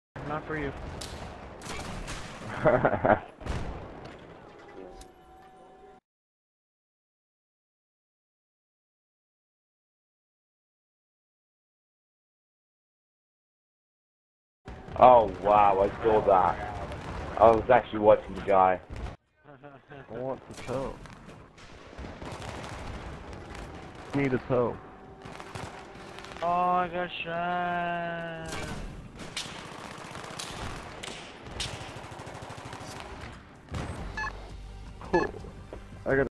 Pro cape kornet launcher
Not really anything special, I just thought it was funny to get two of these in one game in the same life and area.